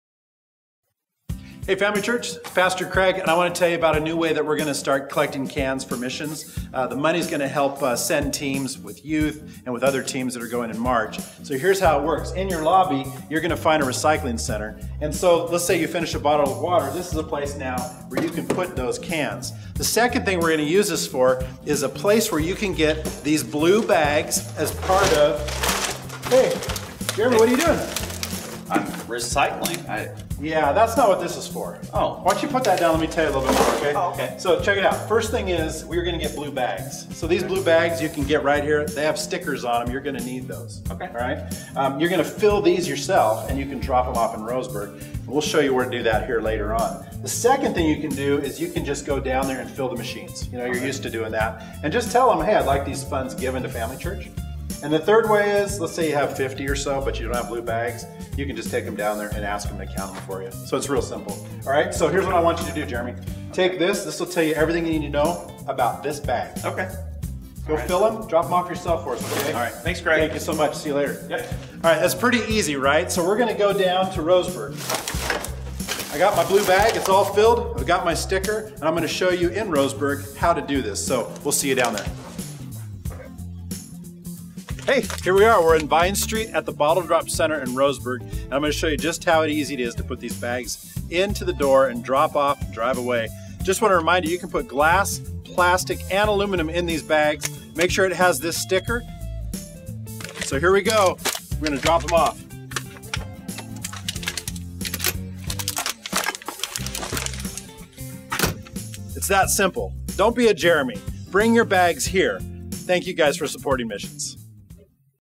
Bottles & Cans to Support Missions!
A new way to donate bottles and cans for FC Missions!
And remember, don't be a Jeremy *wink*